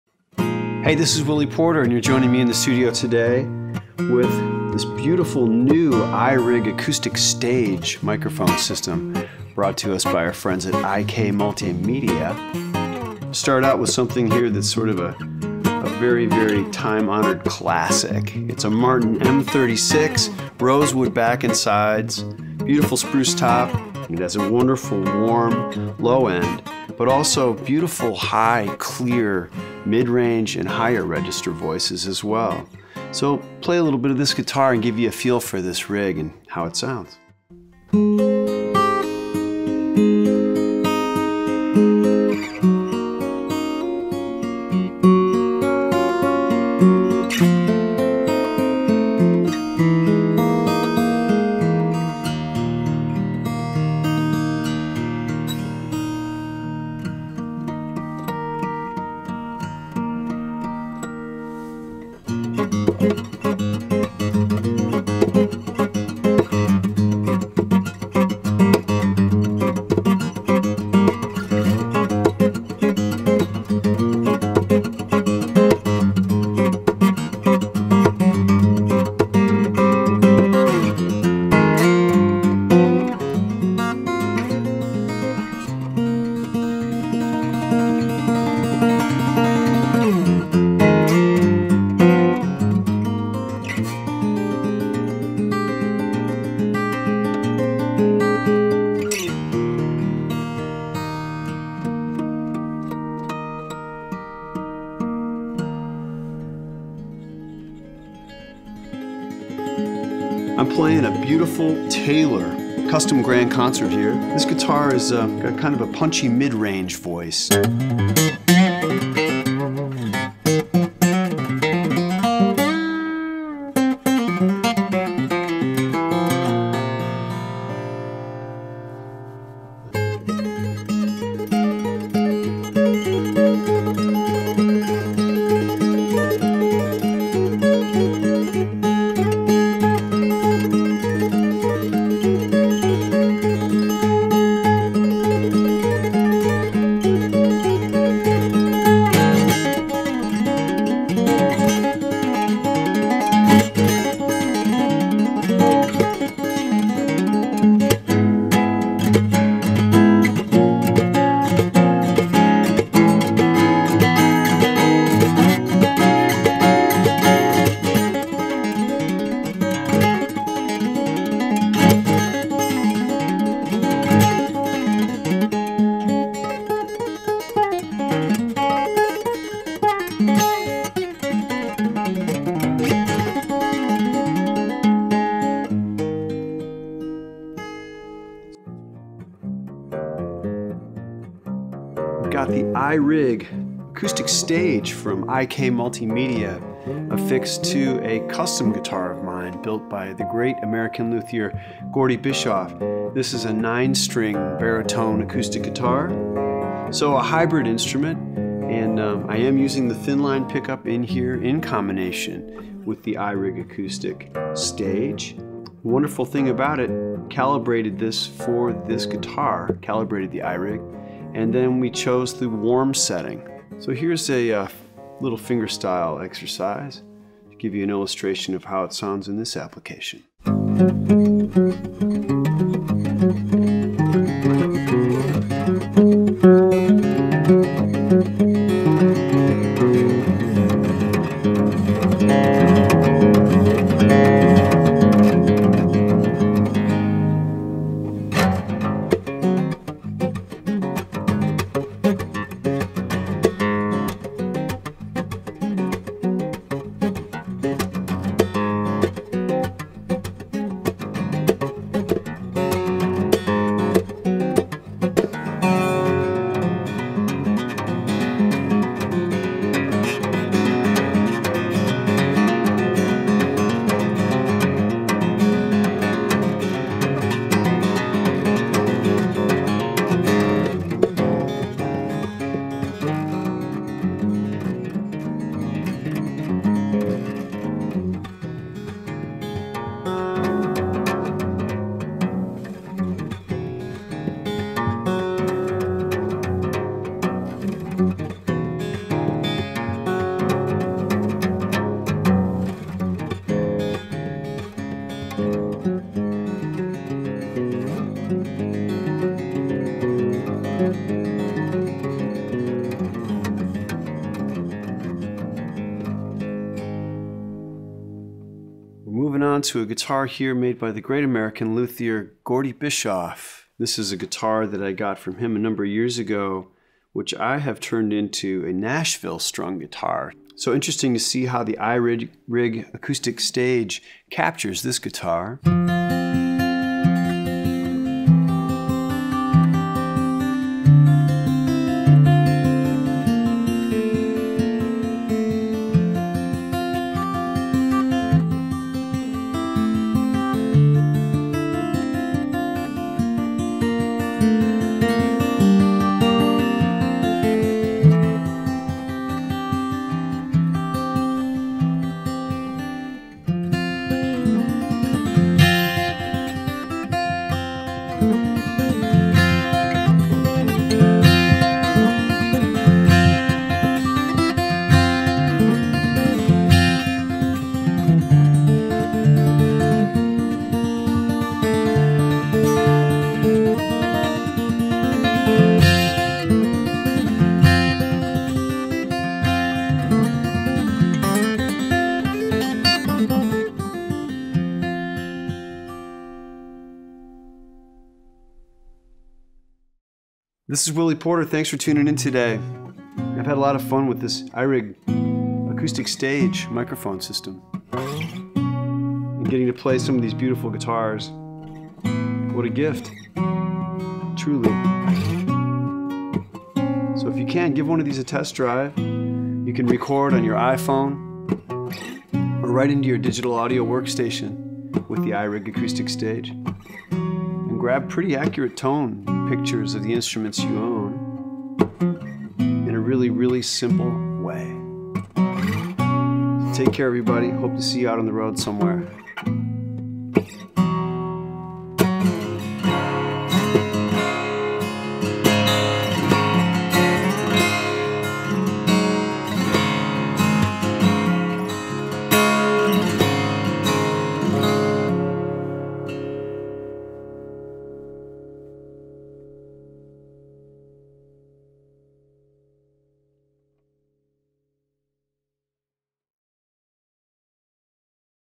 Willy Porter on iRig Acoustic Stage
Watch as the talented Willy Porter uses iRig Acoustic Stage microphone system on various acoustic guitars, and hear how well his incredible playing is captured with such a natural acoustic tone.

Miking your acoustic guitar has become much easier thanks to iRig Acoustic Stage.